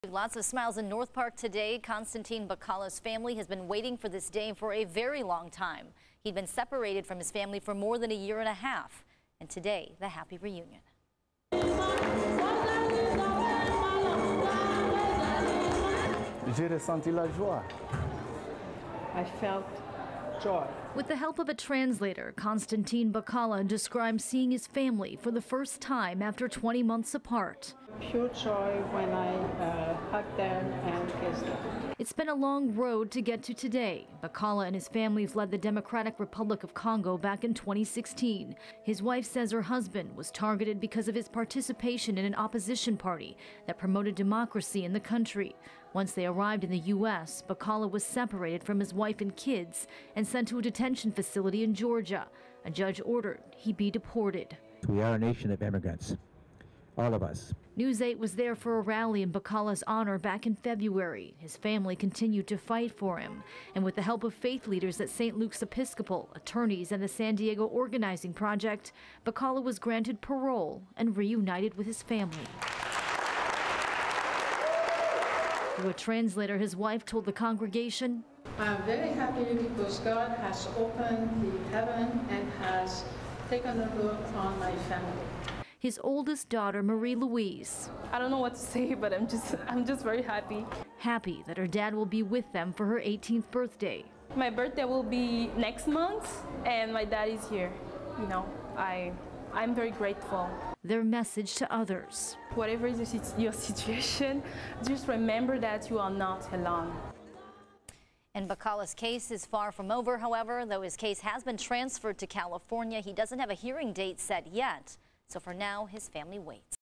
Asylum-seeker joins family in San Diego for ‘welcome home’ party
The culmination of an emotional 20-month journey unfolded in San Diego Sunday, Aug. 25 as a father seeking asylum from political persecution in the Democratic Republic of Congo joined his family in North Park for a “welcome home” party at St. Luke’s Episcopal Church.